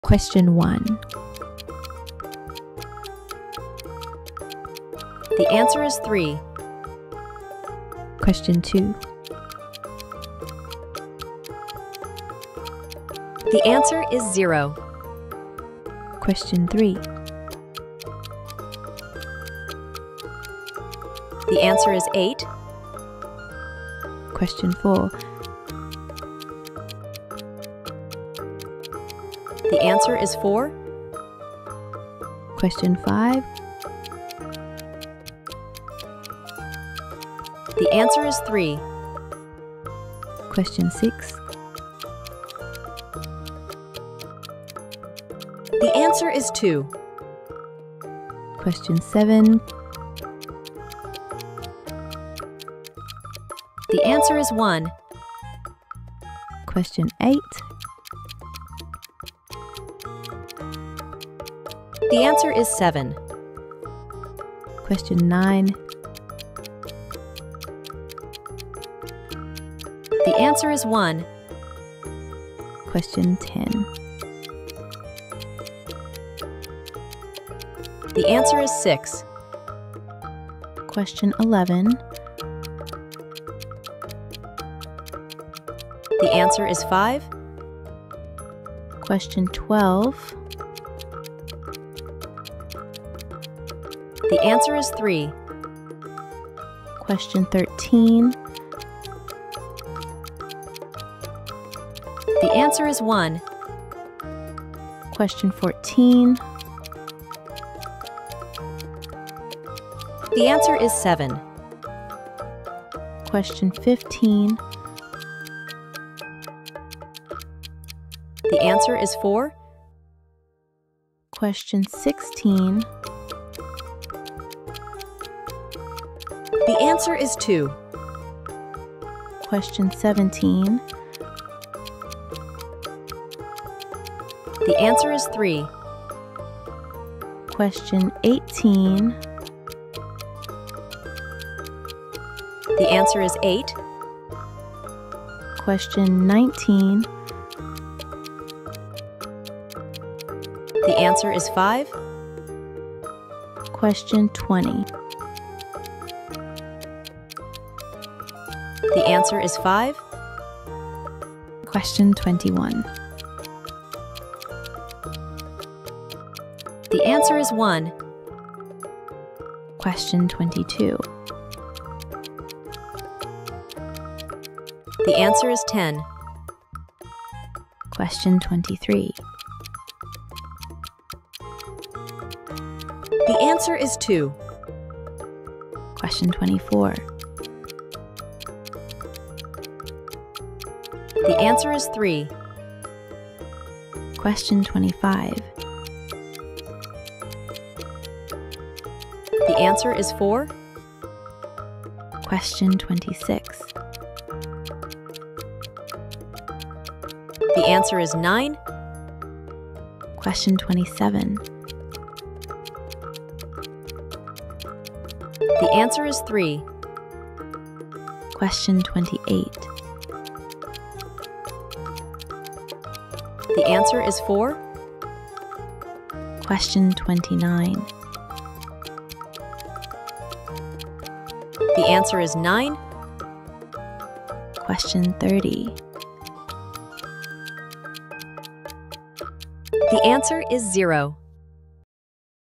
Kindergarten Subtraction Practice | Math Quiz for Beginners
Teach your kids subtraction the fun way! This kindergarten math quiz includes questions up to 10 and helps develop problem-solving skills in young learners.